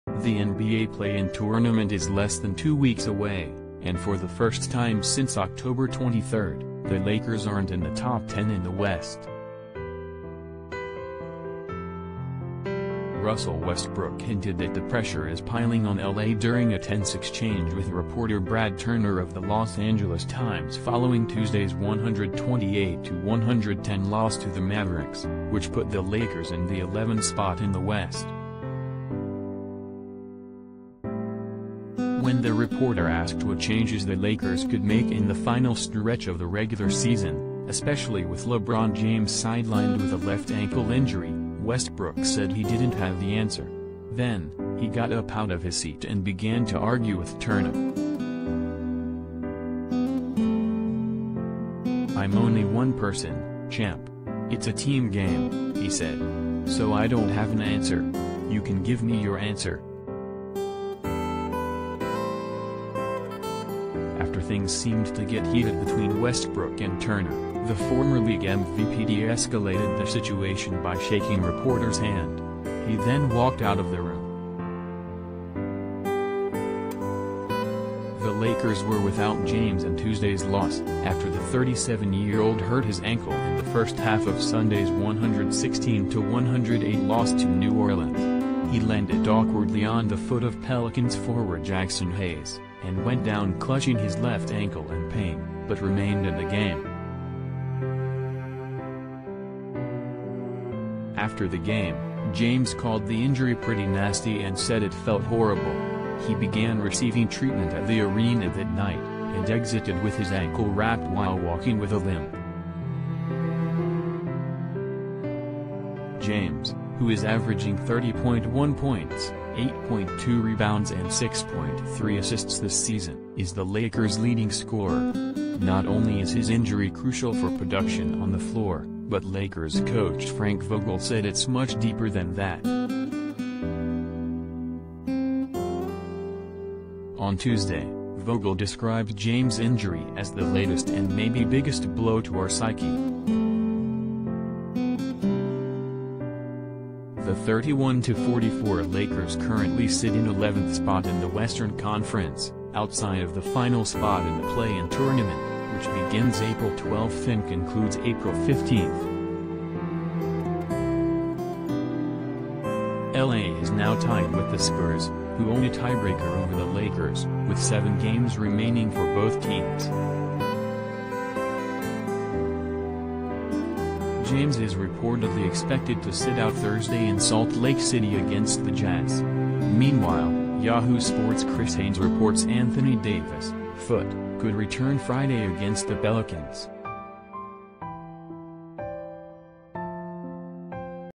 Russell Westbrook snaps at reporter as Lakers search for answers without LeBron James
The NBA play-in tournament is less than two weeks away, and for the first time since Oct. 23, the Lakers aren’t in the top 10 in the West.
Russell Westbrook hinted that the pressure is piling on L.A. during a tense exchange with reporter Brad Turner of the Los Angeles Times following Tuesday’s 128-110 loss to the Mavericks, which put the Lakers in the 11 spot in the West.
When the reporter asked what changes the Lakers could make in the final stretch of the regular season — especially with L